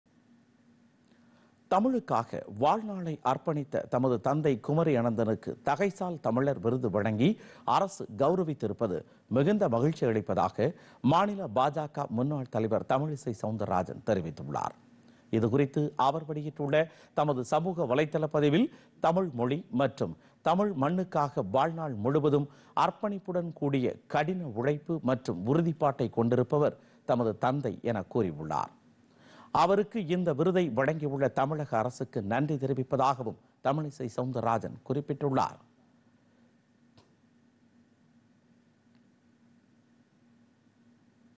KumariAnandhan | குமரி அனந்தனுக்கு தகைசால் தமிழர் விருது - தமிழிசை சவுந்தரராஜன் நன்றி
தமிழுக்காக வாழ்நா‍ளை அர்ப்பணித்த தமது தந்தை குமரி அனந்தனுக்கு தகைசால் தமிழர் விருது வழங்கி அரசு கவுரவித்திருப்பது மிகுந்த மகிழ்ச்சியளிப்பதாக மாநில பாஜக முன்னாள் தலைவர் தமிழிசை சவுந்தரராஜன் தெரிவித்துள்ளர். குமரி அனந்தனுக்கு இந்த விருதை வழங்கியுள்ள தமிழக அரசுக்கு நன்றி தெரிவிப்பதாகவும் தமிழிசை சவுந்தரராஜன் குறிப்பிட்டுள்ளார்